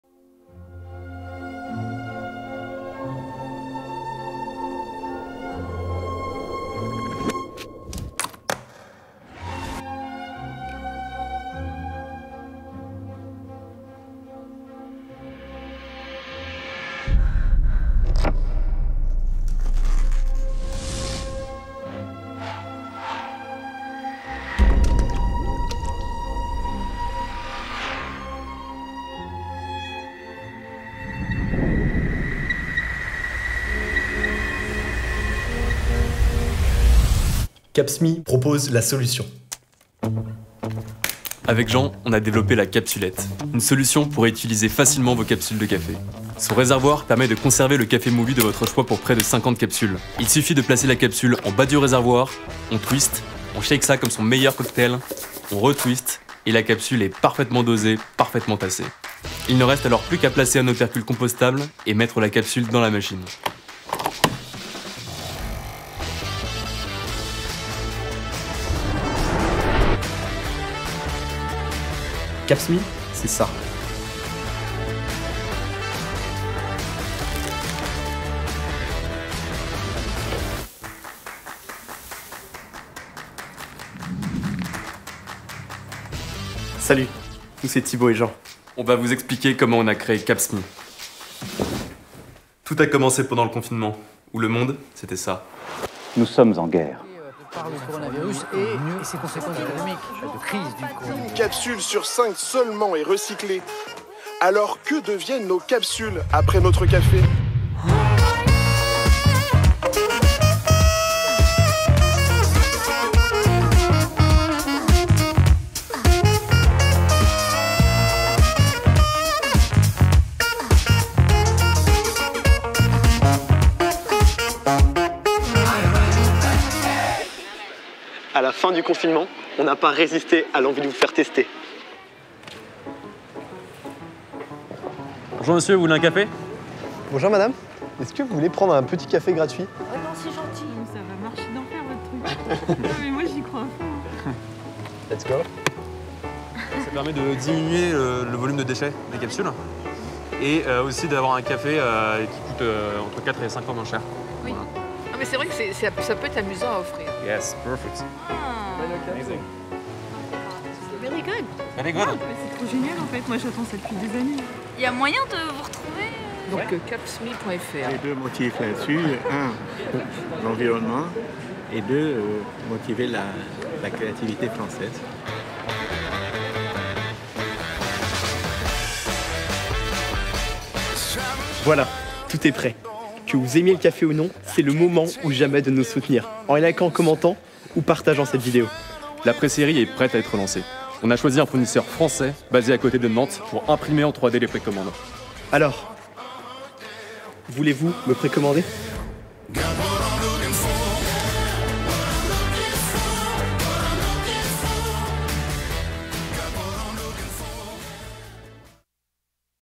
Capsme : le 1er rechargeur de capsules à café écolo
Jean de Boisredon, ingénieur CentraleSupélec, et Thibaut Louvet, des Arts et Métiers, sont fiers de vous annoncer que la capsulette rechargeable écolo CAPSME est désormais en précommande.

Les premiers packs seront livrés avant Noël. N'hésitez à pas à leur faire vos retours.